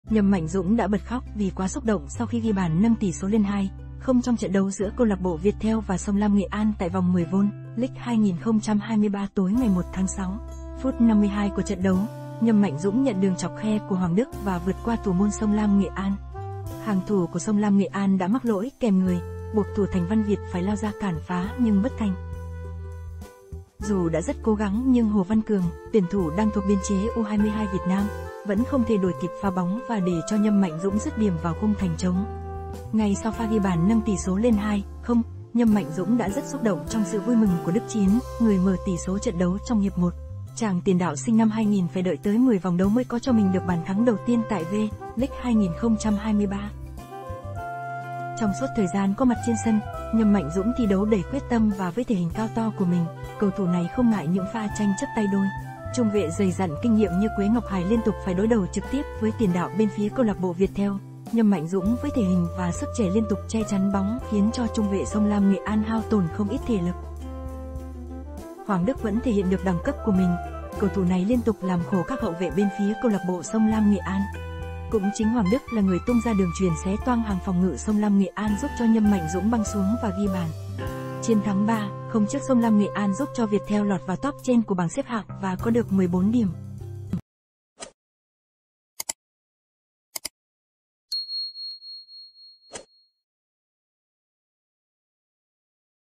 NHÂM MẠNH DŨNG BẬT KHÓC KHI PHÁ LƯỚI SÔNG LAM NGHỆ AN
Nhâm Mạnh Dũng đã bật khóc vì quá xúc động sau khi ghi bàn nâng tỷ số lên 2-0 trong trận đấu giữa câu lạc bộ Viettel và Sông Lam Nghệ An tại vòng 10 V.League 2023 tối ngày 1 tháng 6. Phút 52 của trận đấu, Nhâm Mạnh Dũng nhận đường chọc khe của Hoàng Đức và vượt qua thủ môn Sông Lam Nghệ An. Hàng thủ của Sông Lam Nghệ An đã mắc lỗi kèm người, buộc thủ thành Văn Việt phải lao ra cản phá nhưng bất thành.